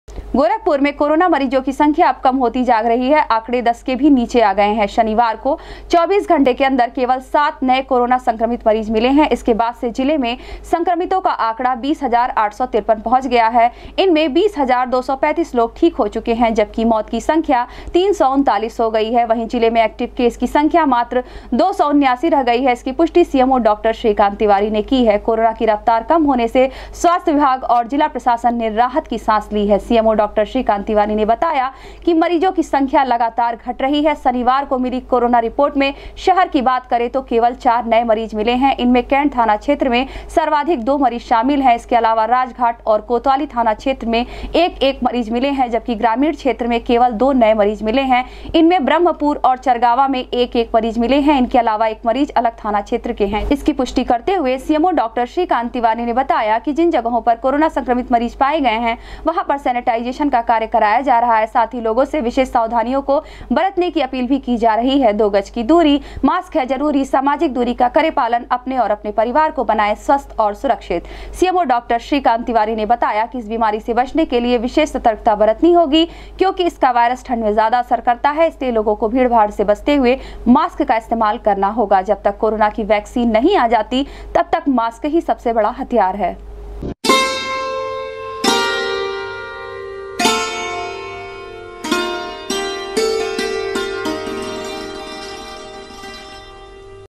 गोरखपुर में शनिवार को मिले केवल 7 नए कोरोना संक्रमित मरीज, जिले में संक्रमित मरीजों की संख्या हुई
गोरखपुर में शनिवार को मिले केवल 7 नए कोरोना संक्रमित मरीज, जिले में संक्रमित मरीजों की संख्या हुई 20853, अब तक 339 कोरोना संक्रमित मरीज गवां चुके हैं अपनी जान, मात्र 279 रह गए एक्टिव मरीज, सीएमओ डॉ श्रीकांत तिवारी ने की पुष्टि